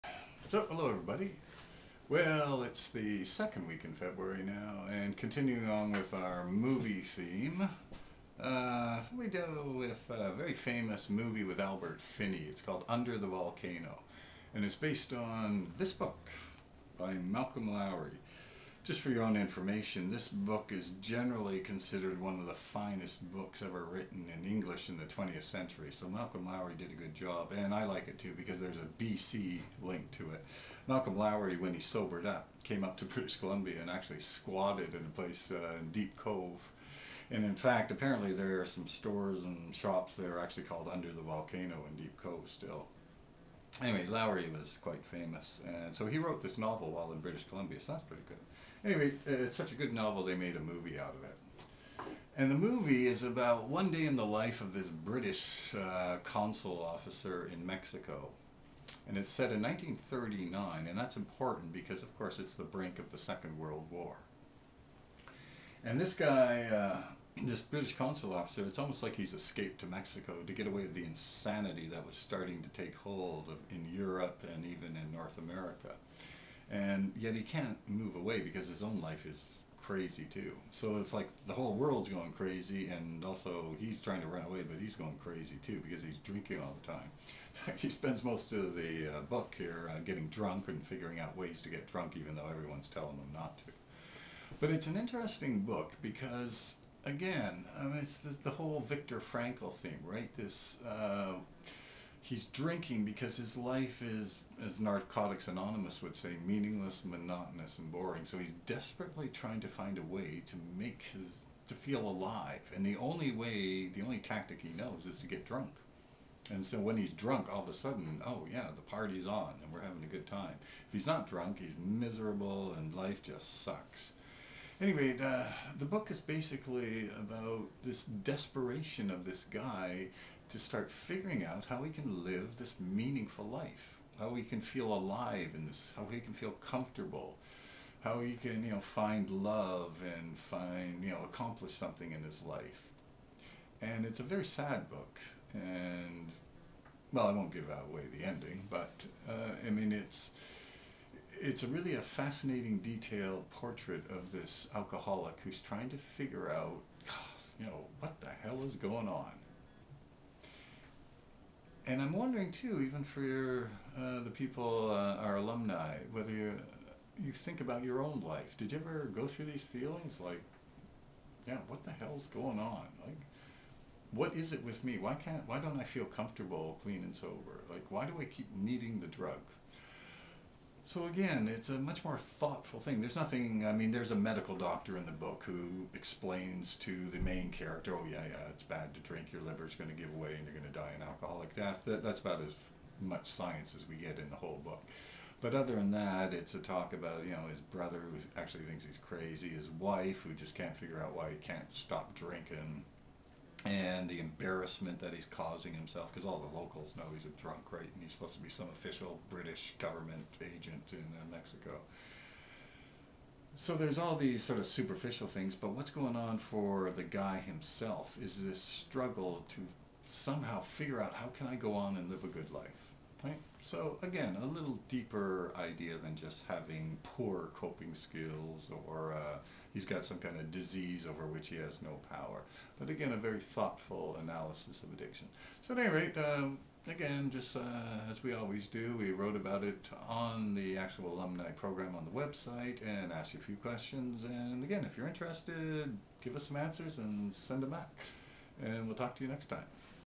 Addiction & Recovery: Under the Volcano
Geoff Thompson, SCHC Clinical Director, discusses the quest for a meaningful life and how this search is portrayed in the movie "Under the Volcano" based on the book written by Malcolm Lowry.